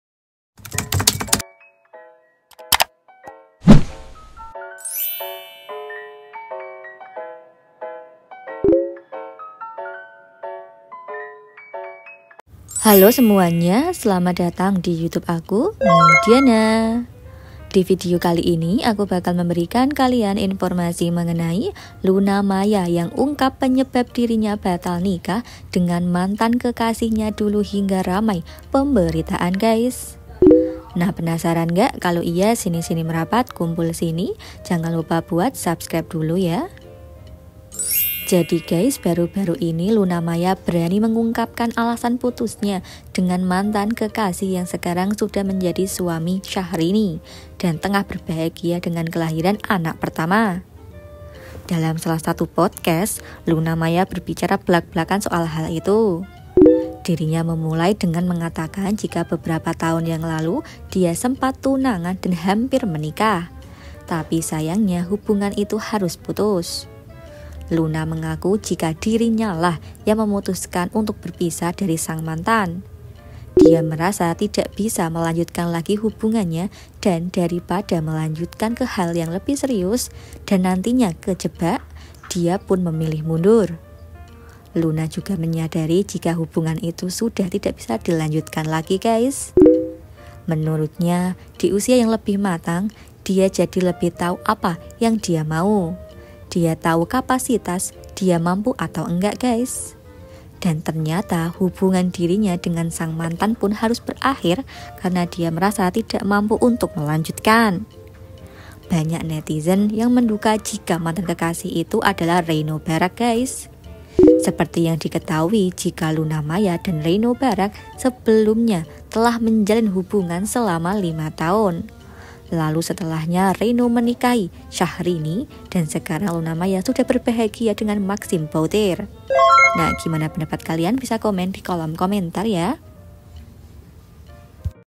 LUNA MAYA UNGKAP ALASAN GAGAL NIKAH SAMA REINO BARACK!! BLAK-BLAKAN SEBUT HAL INI!!
LUNA MAYA UNGKAP ALASAN SEBENERNYA PUTUS SAMA REINO BARACK PADAHAL MAU NIKAH